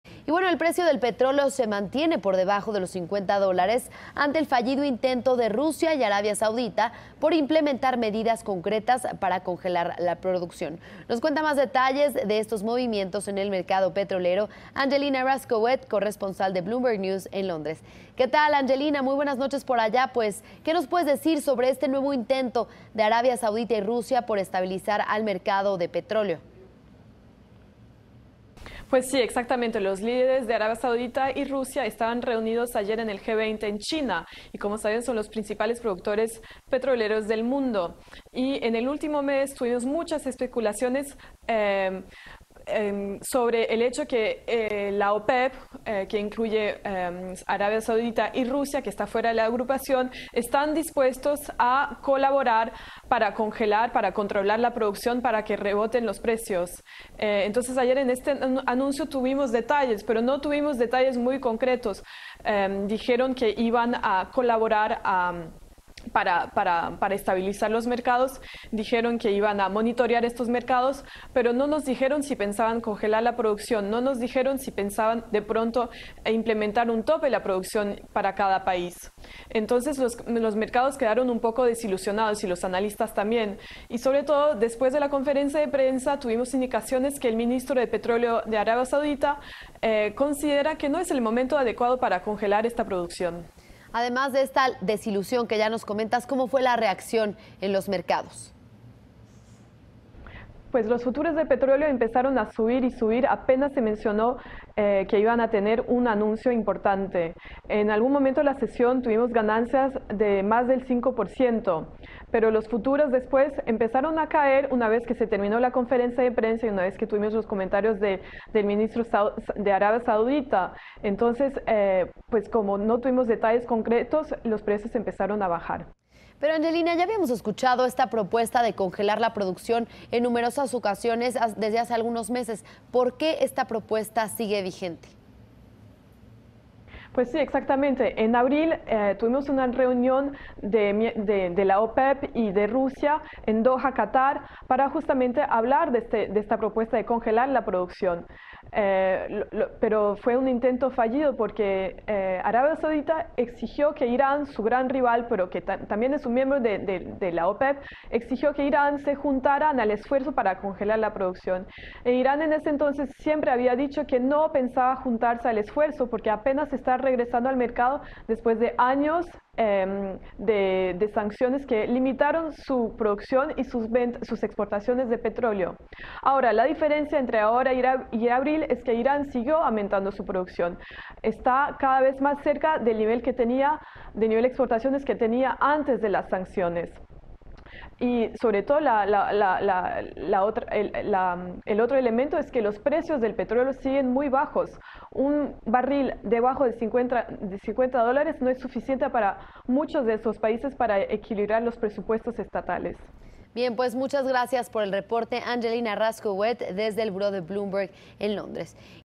Mercados decepcionados por fallido intento de congelar producción de petróleo de Arabia y Rusia
Angelina Rascouet, corresponsal Blomberg Londres, conversó con Susana Sáenz fallido intento de Rusia y Arabia Saudita de congelar la producción del petróleo en la cumbre de la OPEP.